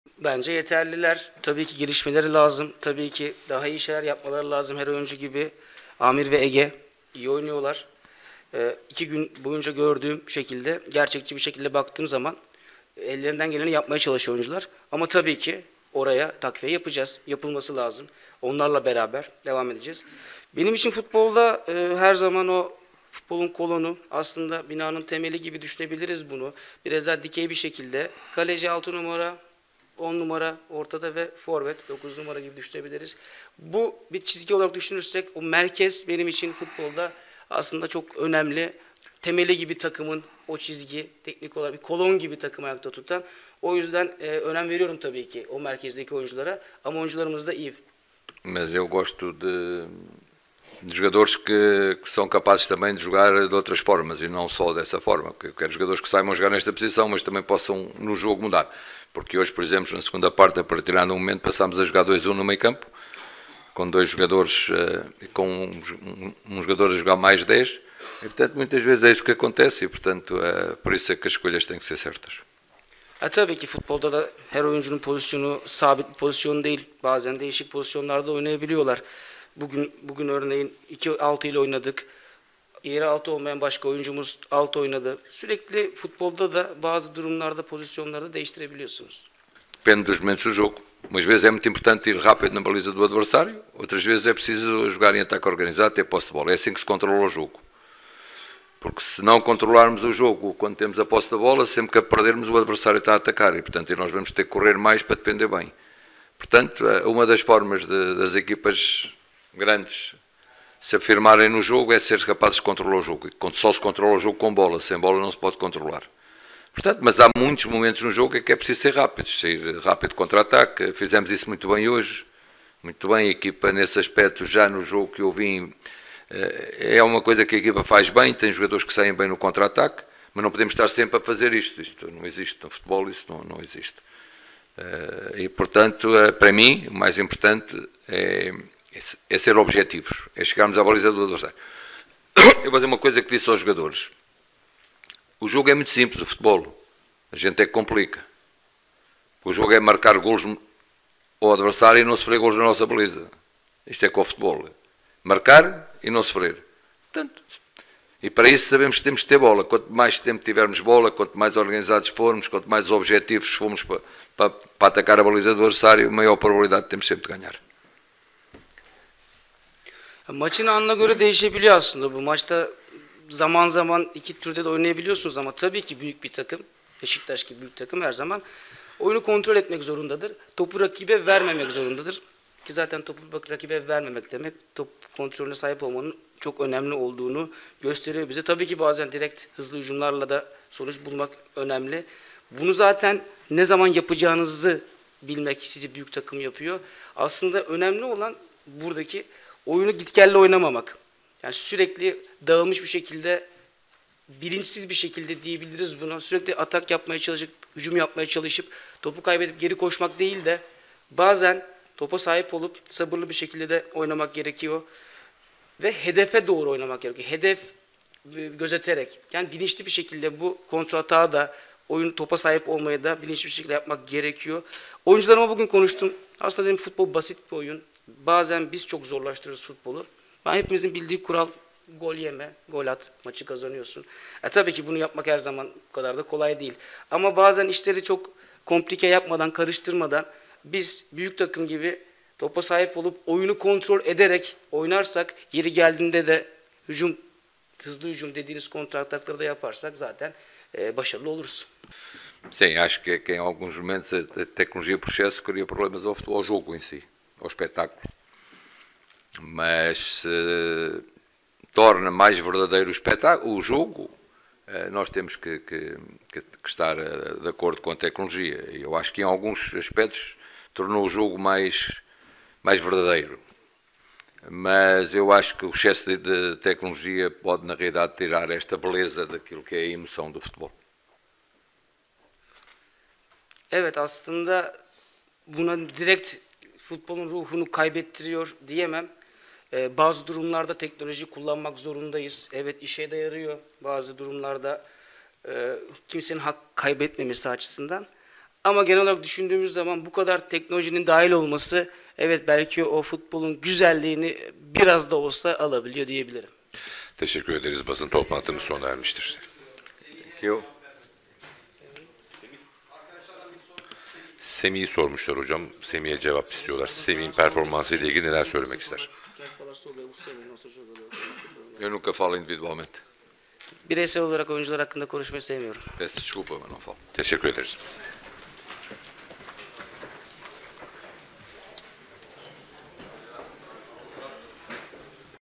Fernando Santos'tan transfer itirafı! Basın toplantısında açıkladı
Trendyol Süper Lig'in 20. haftasında Beşiktaş ile VavaCars Fatih Karagümrük karşılaştı. Beşiktaş'ın yeni teknik direktörü Fernando Santos maçın ardından basın toplantısı düzenledi.